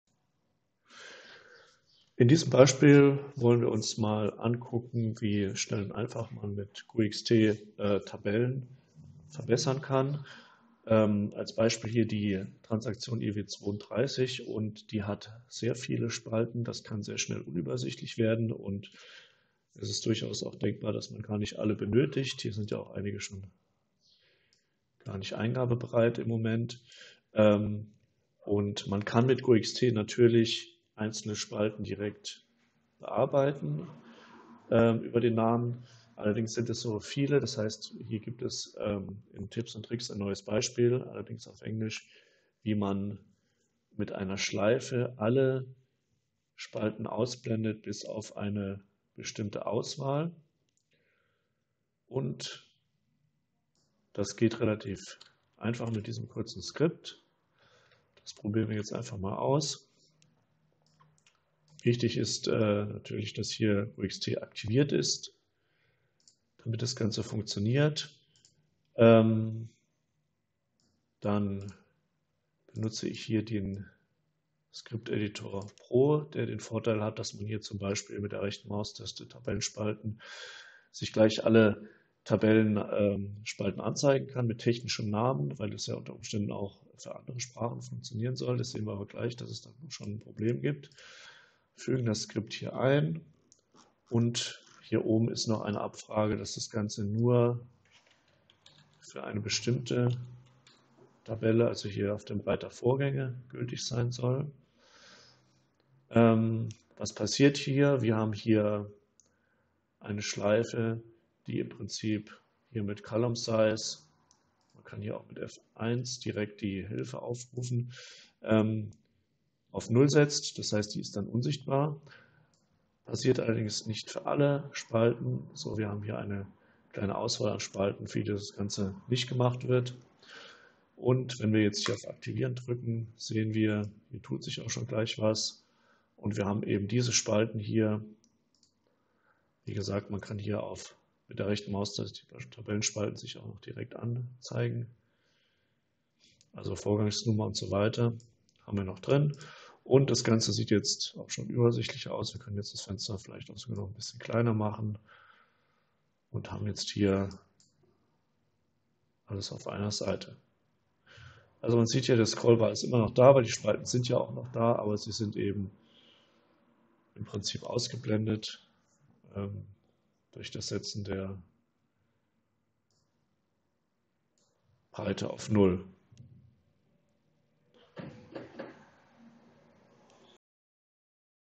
SAP GUI 7.60 - IW32 Tabellenspalten anpassen mit GuiXT
Unterdrückt alle Spalten in einem Table Control mit Ausnahme einiger weniger Spalten.

Verwenden Sie die GetFieldAttribute-Anweisung in einer Schleife über alle Spalten.

Beispiel:
In der Transaktion IW32 (Serviceauftrag ändern) werden die Vorgänge des Serviceauftrags in einem Table Control mit 89 Spalten angezeigt:

Für eine bestimmte Anwendung wollen wir nur 6 Spalten anzeigen, damit der Benutzer nicht horizontal blättern und die benötigten Spalten suchen muss.